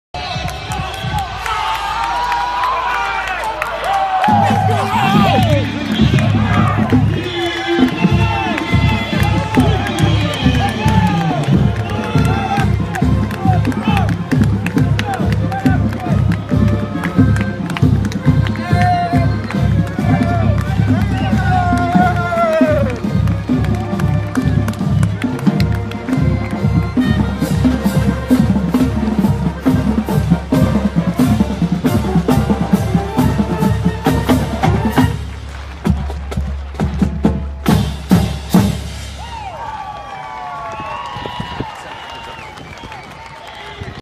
Indiana State football takes the field for the Sycamores' game against Butler at Memorial Stadium.
By: Joseph C. Garza
Published on: September 3, 2016